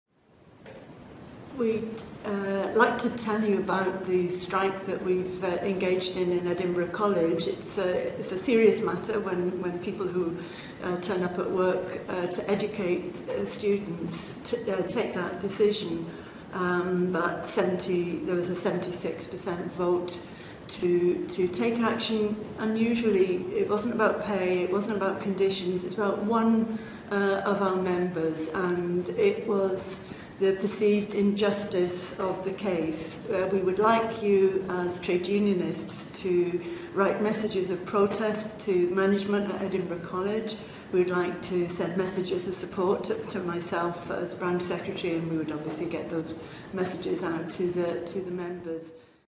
Edinburgh College Industrial Action - Message of Support
EIS Edinburgh Branch Secretary Penny Gower encourages members to show support during Edinburgh College Industrial Action.